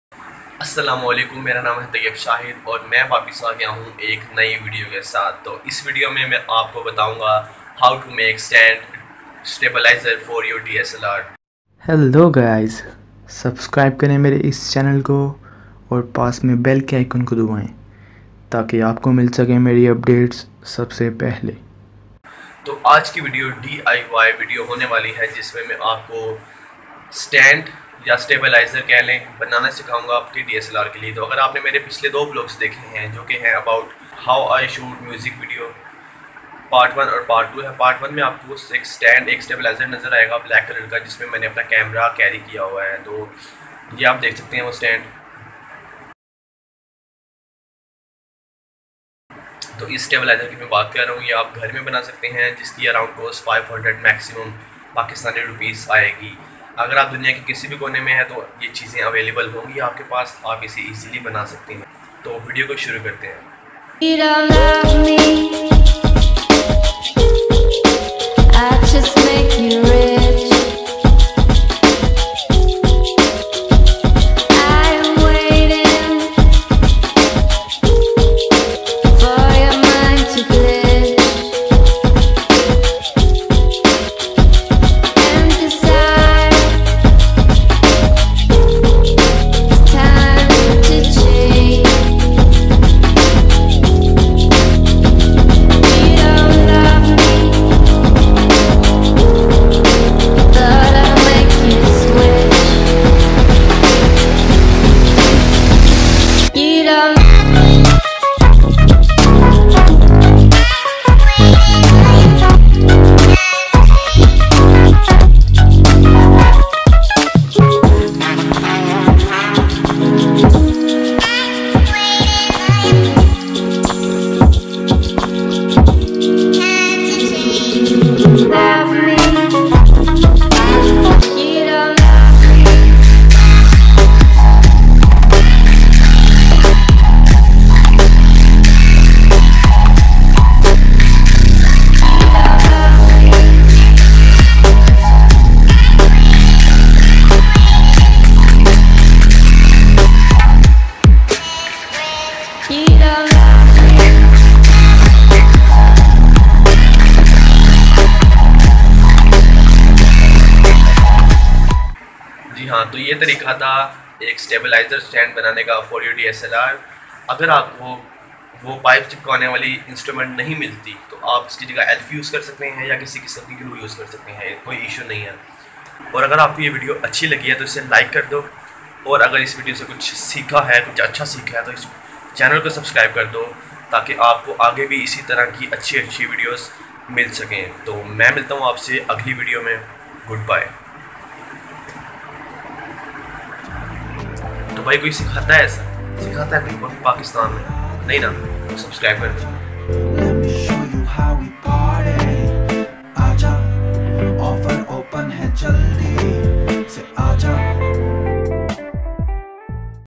How To Make Stabilizer For DSLR At Home DIY
Hello
My name is tayyab shahid and in this video im showing you how to make styabilizer at home for your DSLR wihitin just 500 pakistani rupies
THANKS!

4:Snapchat :tayyabhash2